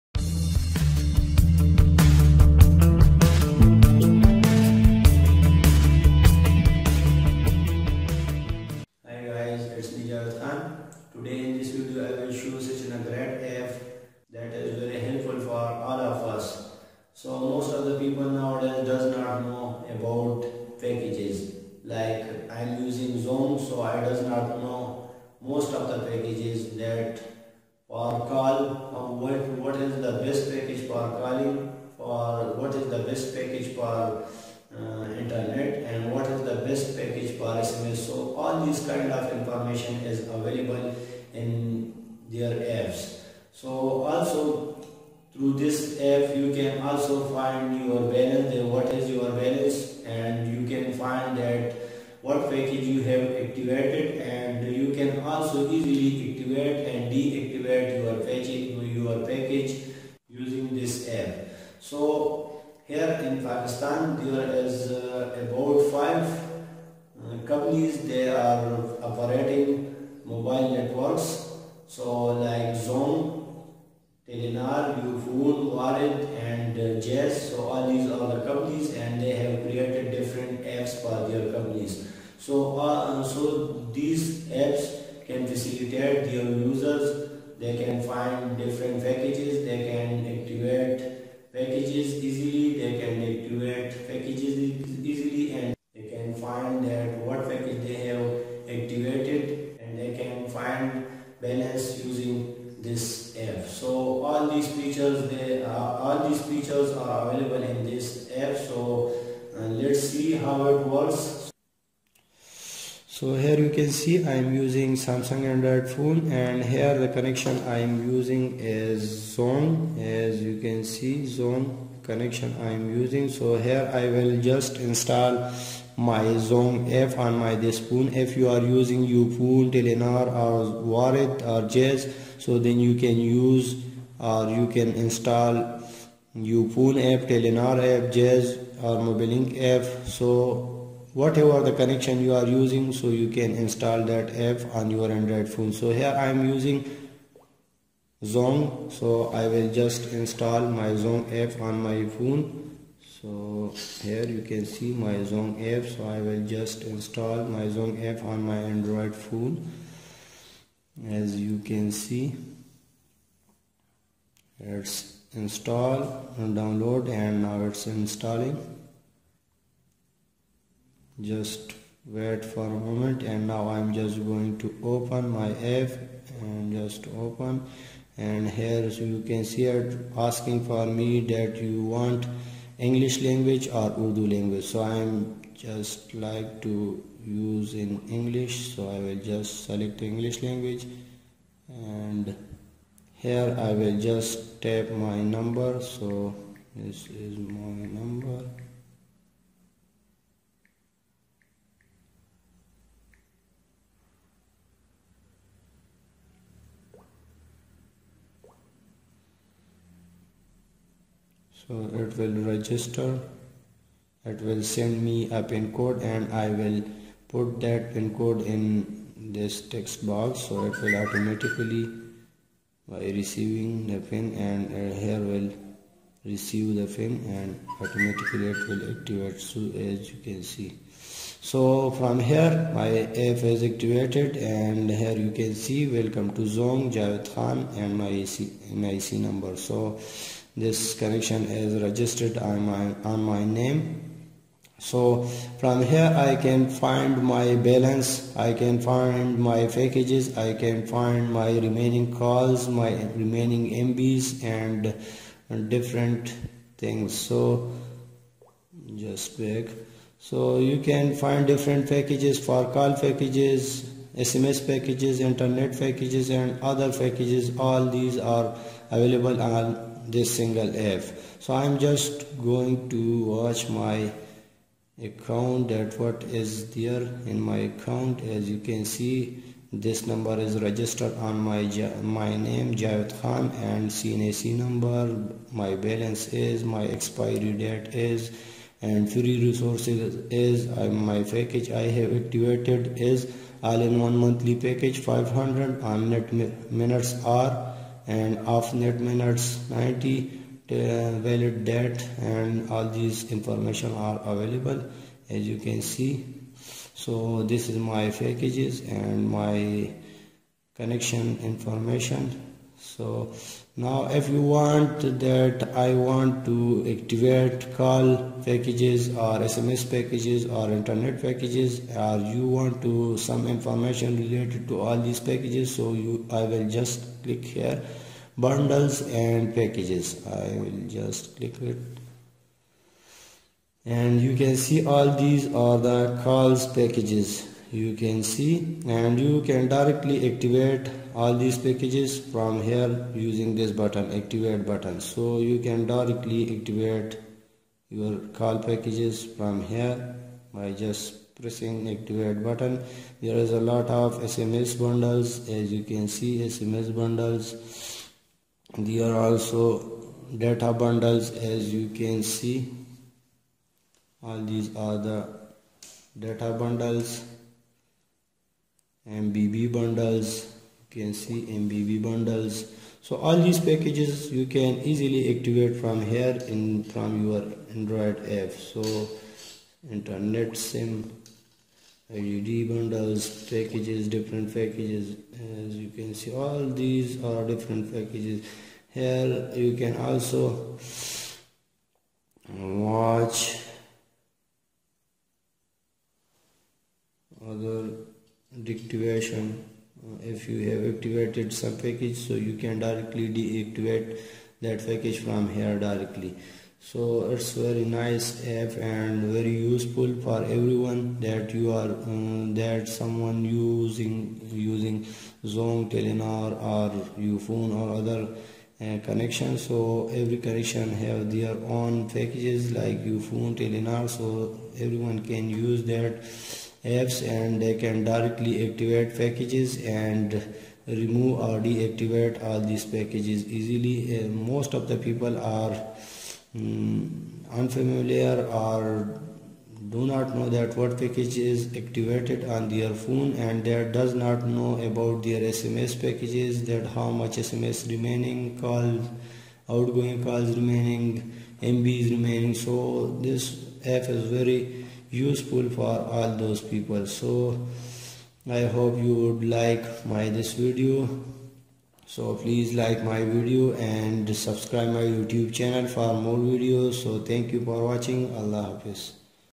How to Check activate Call SMS Internet Packages in Zong Telenor Ufone Jazz warid Using Android App
Hi Guys, In this video i will show you that How to Check activate and Deactivate Call, SMS, Internet, Packages in Zong, Telenor, Ufone, Jazz warid using Android App. There is a lot of information about you connection. You can check sim detail package detail using this app.